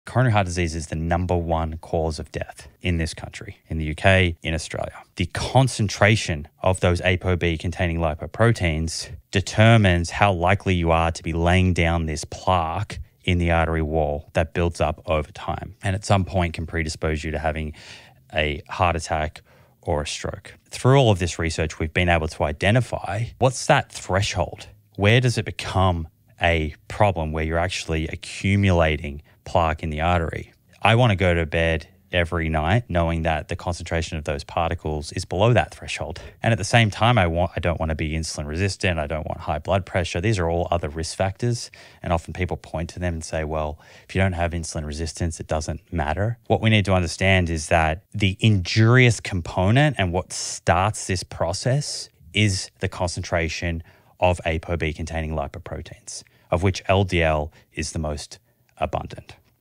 The REAL Culprit in Heart Diseases?! | Simon Hill and Mari Llewellyn | Shorts
ApoB lipoproteins are tiny particles in your blood that can cause big problems for your heart. I explain here why they might be more important than LDL cholesterol. Ever wondered what actually kicks off heart disease? Let’s break it down and touch on how things like smoking and alcohol play into it all.